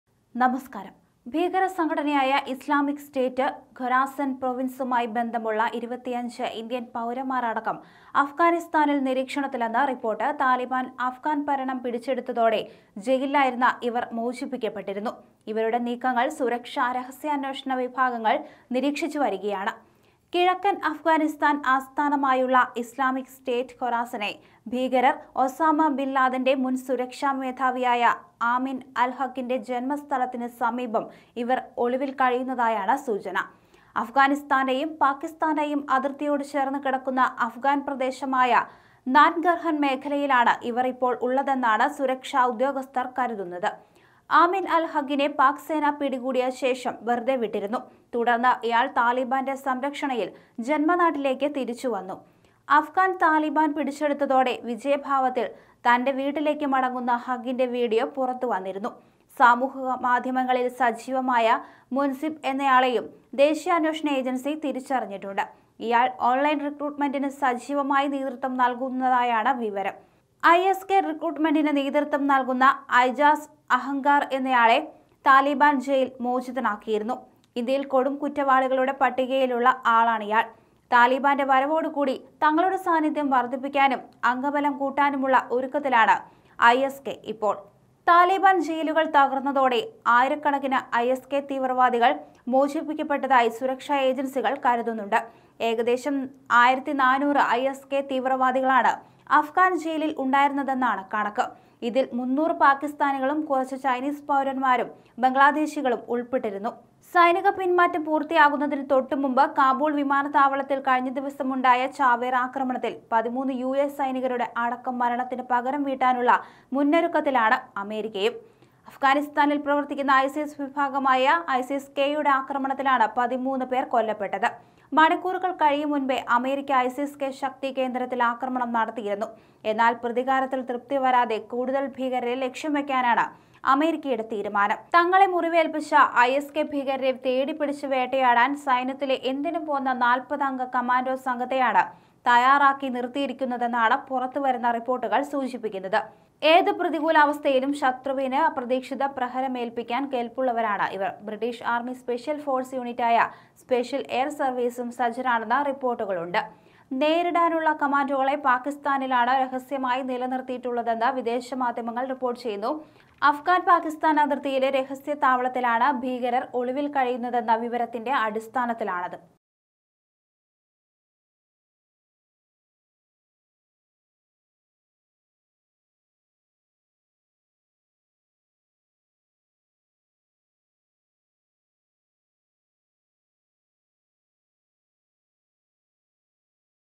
ഐസിസ് കെ ഇന്ത്യന്‍ സംഘം ഒരുമിക്കുന്നു I Afghan news updates
താലിബാന്റെ വരവോടെ അഫ്ഗാനിൽ നിലയുറപ്പിക്കാൻ ഐ.എസ്.-കെ; ജയിൽ മോചിതരായവർ 'ഒന്നിക്കുന്നു'; 25 ഇന്ത്യക്കാരടക്കം രഹസ്യാന്വേഷണ വിഭാഗങ്ങളുടെ നിരീക്ഷണത്തിൽ; കിഴക്കൻ അഫ്ഗാനിലെ ശക്തികേന്ദ്രത്തിൽ തിരിച്ചടിക്കാൻ അമേരിക്ക; ഡൈഹാർഡ് സ്‌ക്വാഡിനെ നിയോഗിച്ചതായി റിപ്പോർട്ട്